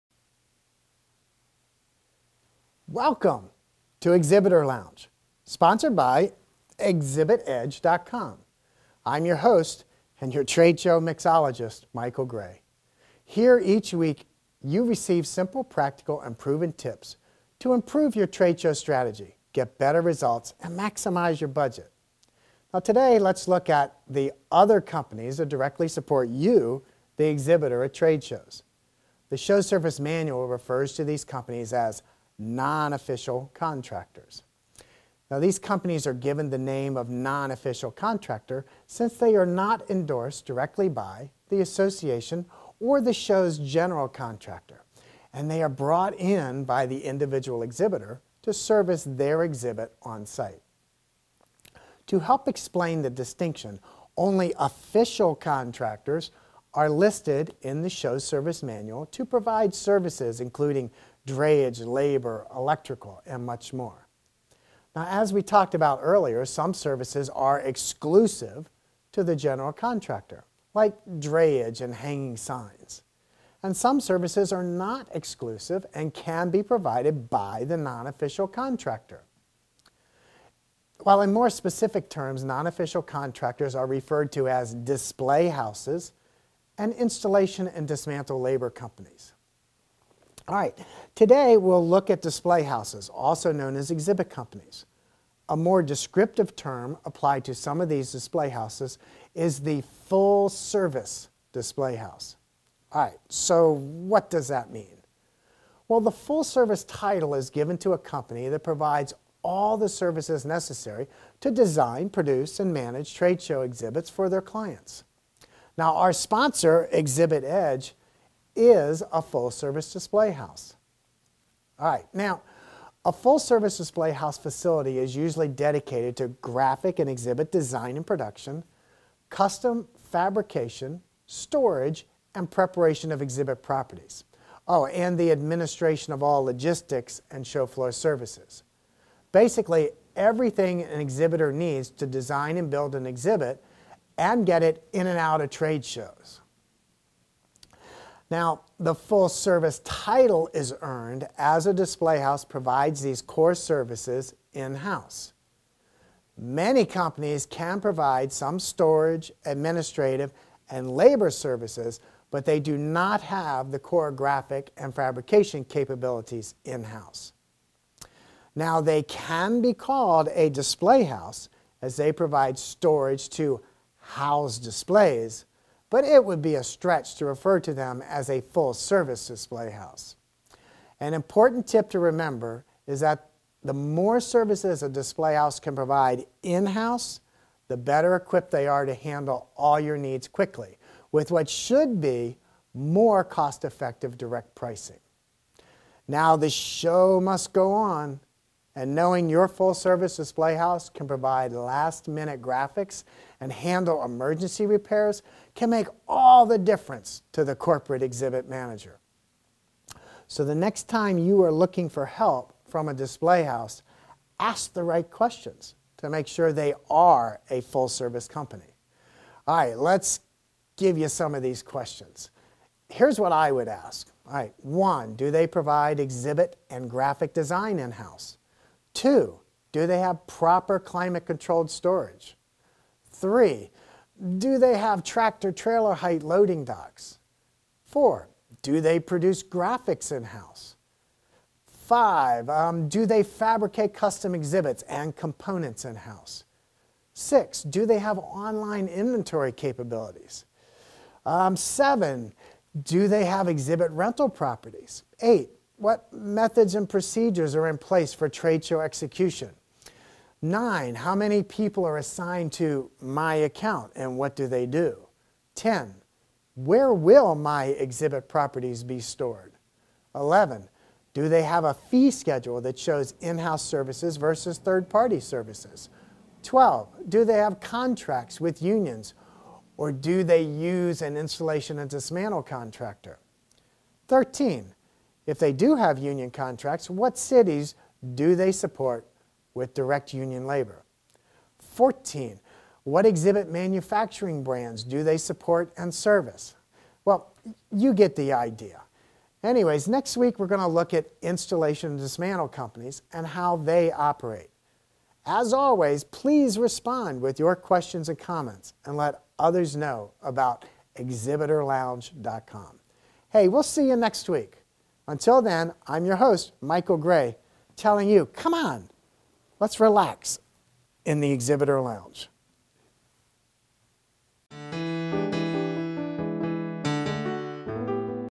What is a Full Service Display House
- Hello fellow Exhibitors! In this weeks episode we discuss what sets "Full Service" Display Houses apart from the status quo and what questions you should ask, in order to, vet a company as a Full Service Display House.